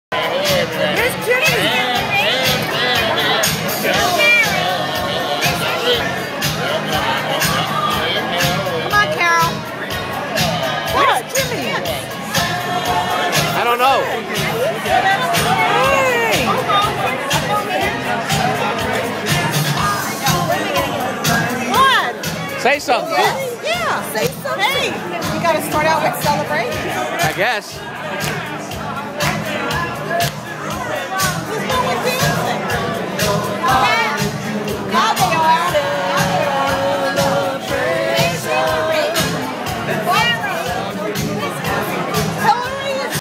Far Rockaway High School Reunion
At the table at the Far Rockaway High School Reunion, Deerfield Beach, FL.  It is August 28th 2010.